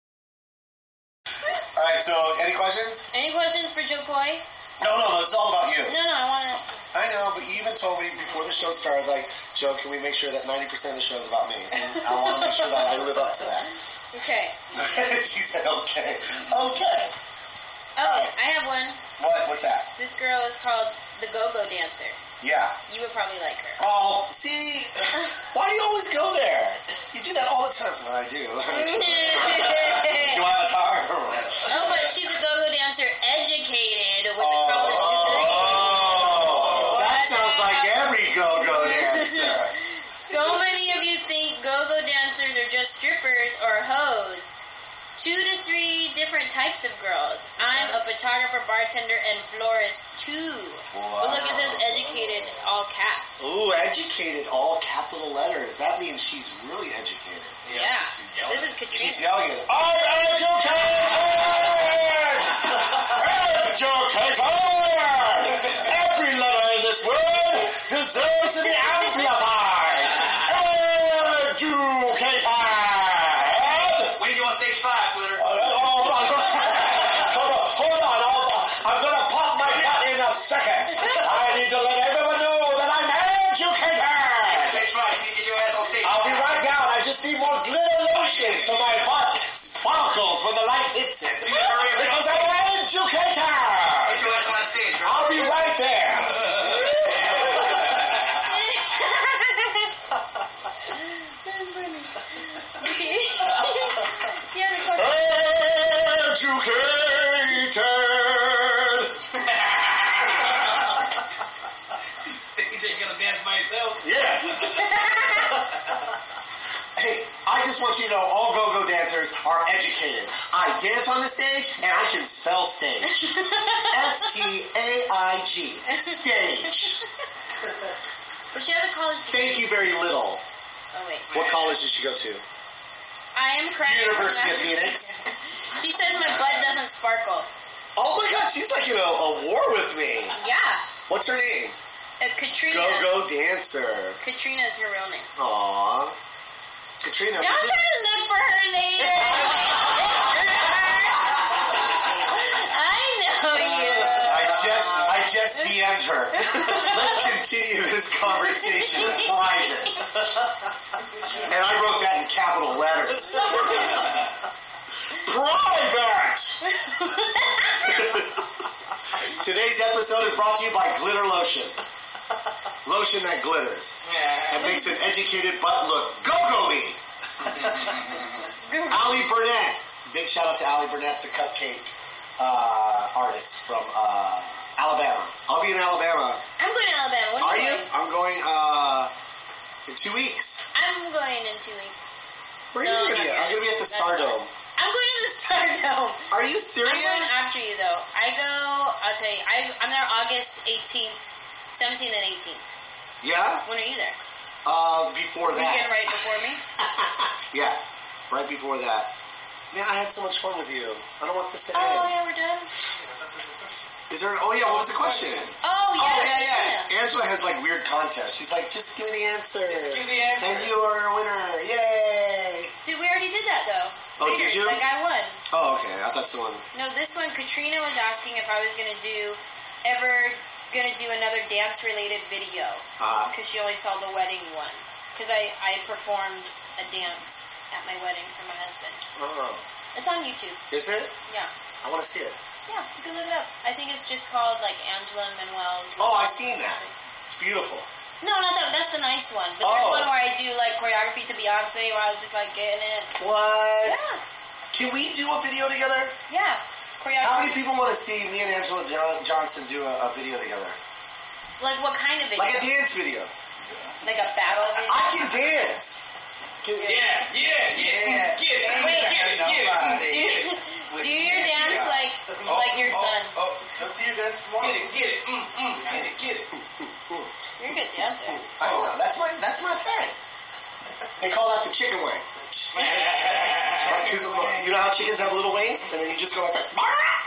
Jo Koy, Anjelah Johnson, GoGo Dancers & My Twitter
I tuned into their show because I find these 2 effing hilarious on their own.  It would only be even more epic to see them together!  So my brother and I watched the show :).  They asked watchers if we had questions using Twitter so of course I asked about dancing to Anjelah, and they ended up reading it through the show.

If you ever see my Twitter profile it sounds show-offy lol (I realized it even more especially how they read it), but it's actually meant as a reaction to those who think of gogo dancers as strippers, hoes, sluts, uneducated, or high school drop outs kind of thing.  All I have to say is that it's a goooood thing it didn't say that I graduated from nursing school OR that I'm Filipino OR that I live in both Houston, TX and California because I assume they would have a field day with all that information. I can only laugh!    Put some "glitter" in your day and look up their previous stand up comedies on youtube if you've never heard them :).

I tend to like people whether I've met them in real life or not

I hope this is legal!
Follow either of them on Twitter :) @jokoy or @anjelahjohnson or myself @thegogodancer (hahaha which I changed bc it initially read "this"gogodancer until Anjelah used her own words when she read it lol).